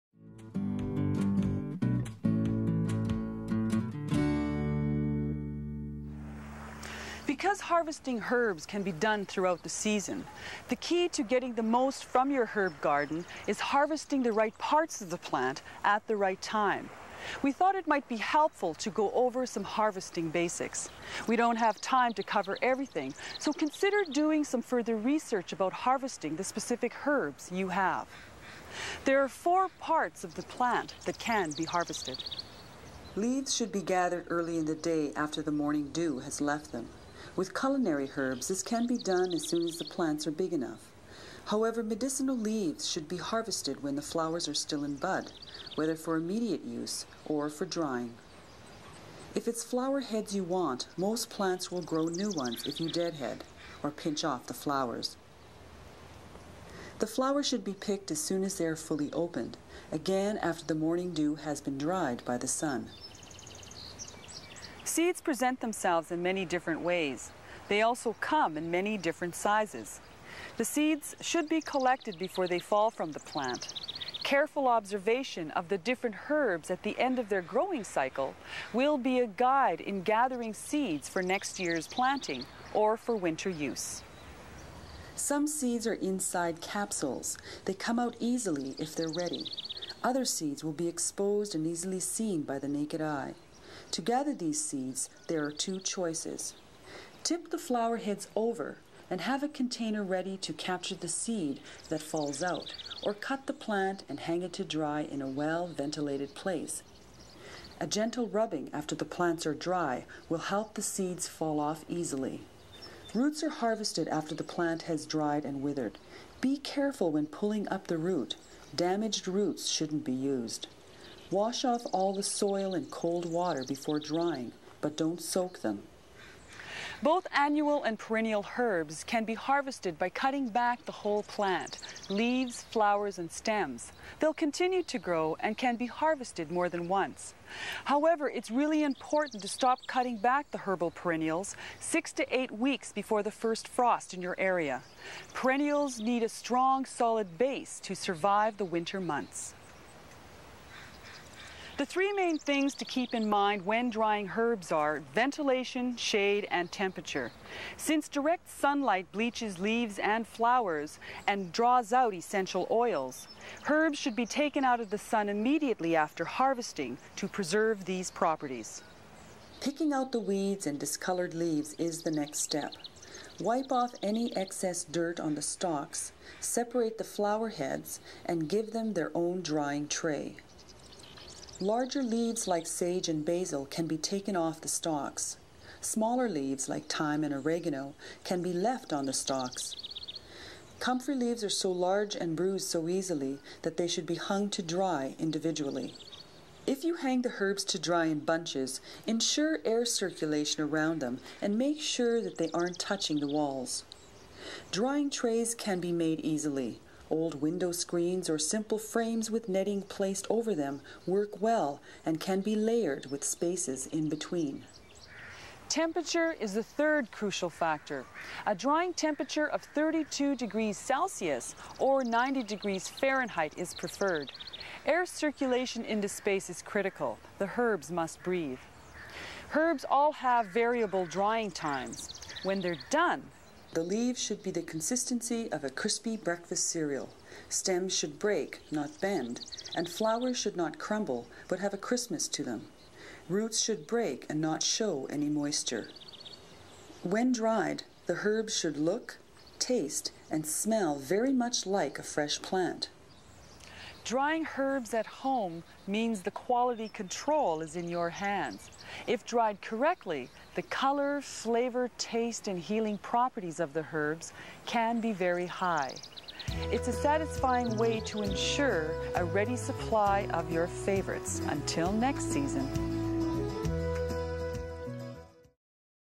Harvesting Herbs Sustainably
How to get the most from herbs and make sure you can use your seed for years to come. This video shares how to harvest.